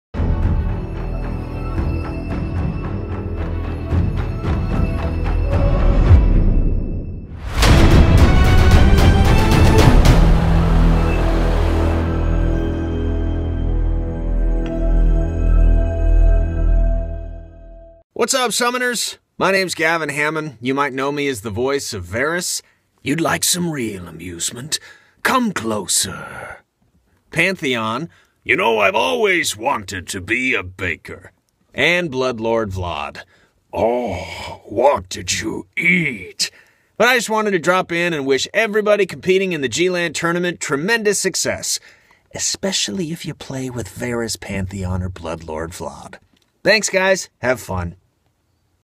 G-LAN
DescriptionGavin Hammon drops on by to wish all the teams competing at our Summer G-LAN championship the best of luck!